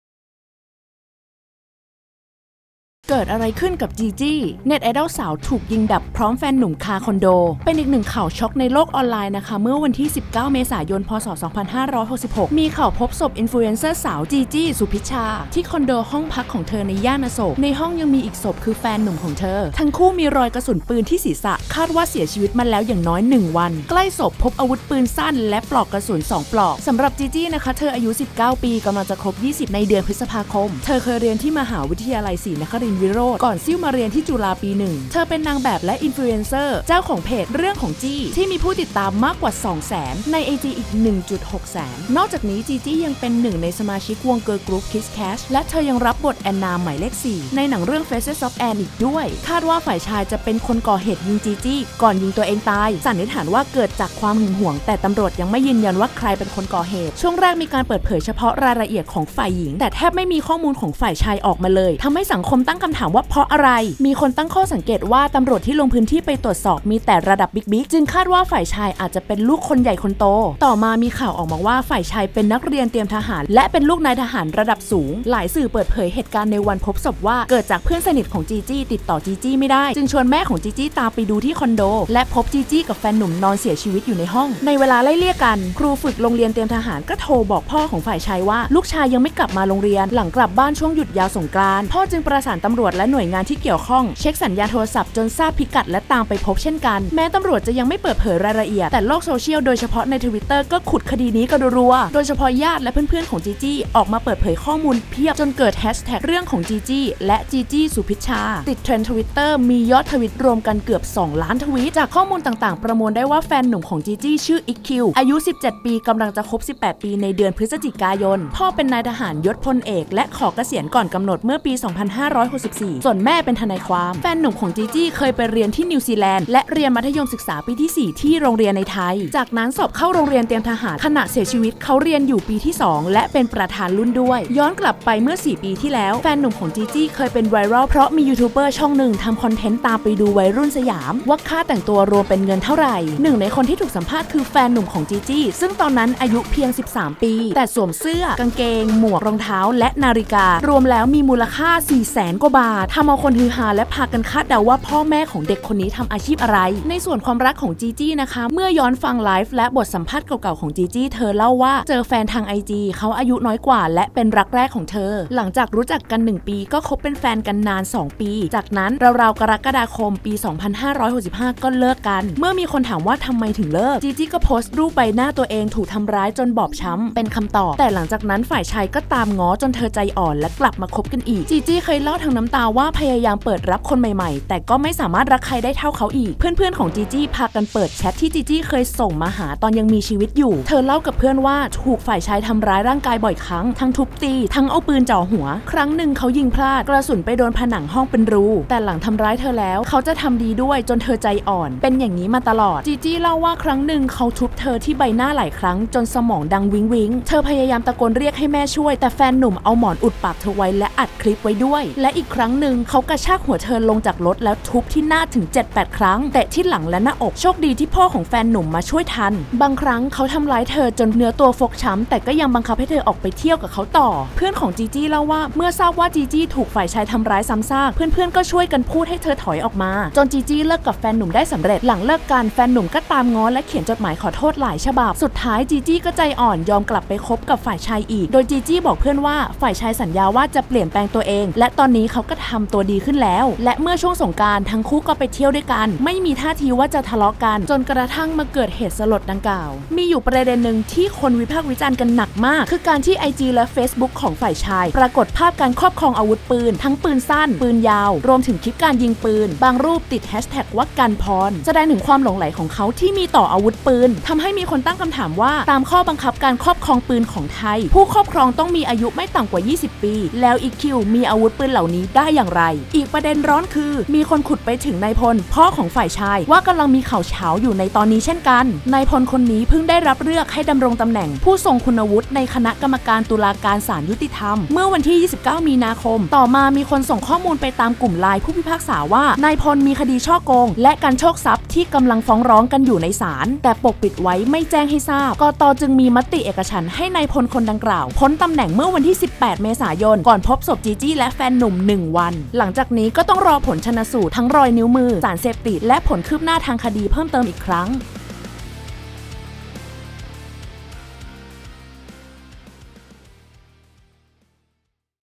เกิดอะไรขึ้นกับจีจี้ : [News Story]
สรุปแฮชแท็ก "เรื่องของจี้" จีจี้ สุพิชชา ถูกยิงเสียชีวิต
คนใกล้ชิดเปิดแชต ที่ผ่านมาถูกทำร้ายร่างกายโดยตลอด